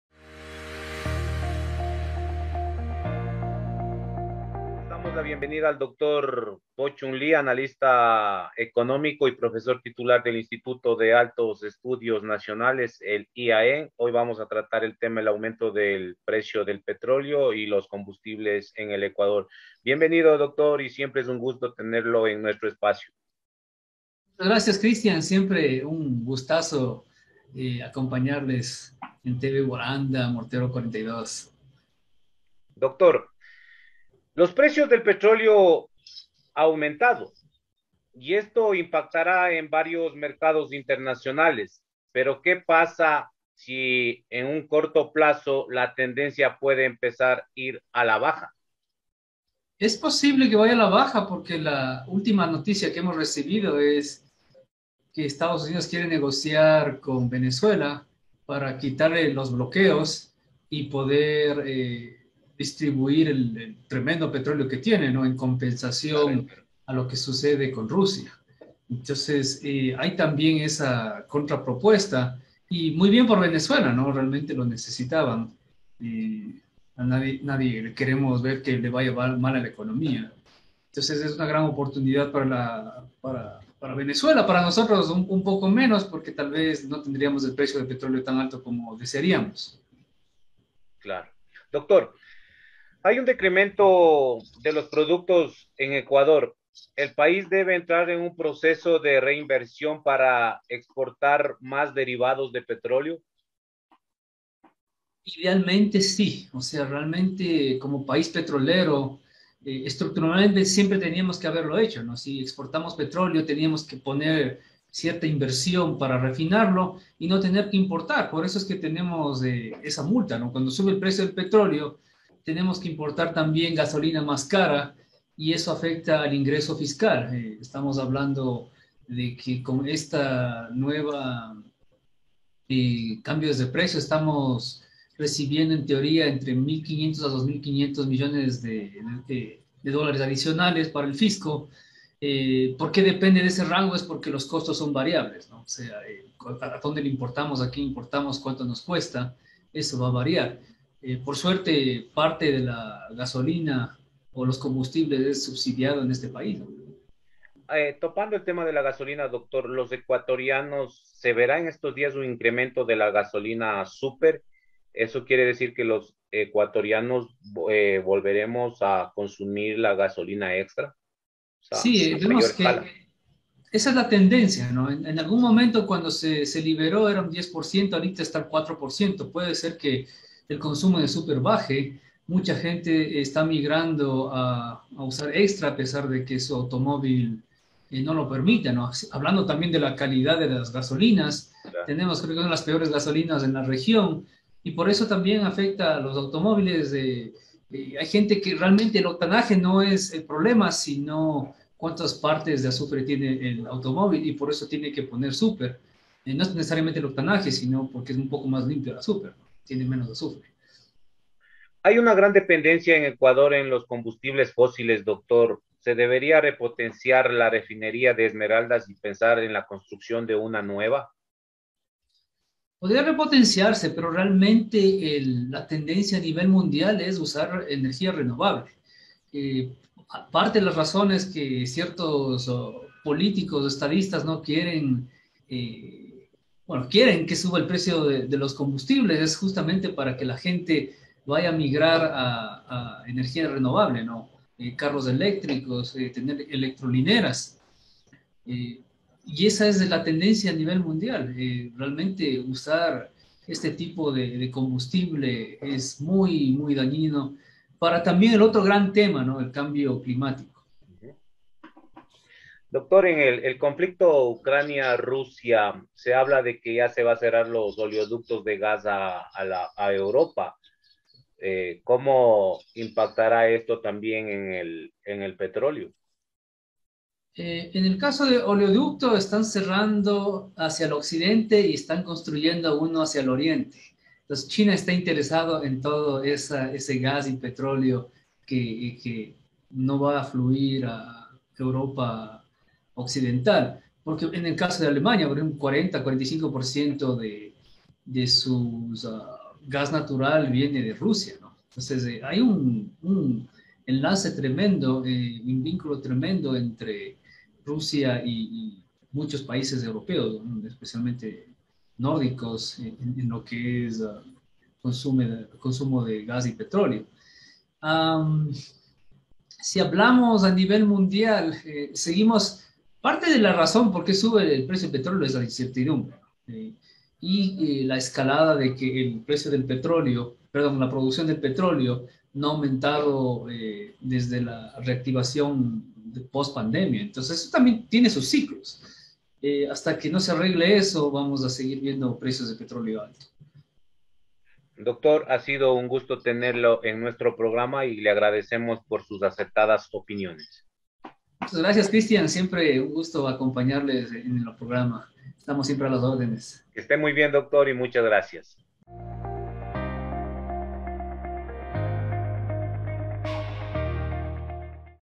Análisis: Impacto real del alza del precio del petróleo en la economía del Ecuador
Entrevista en Guaranda Tv, al Dr. Po Chun Lee, Docente de la Escuela de Economía Pública del IAEN.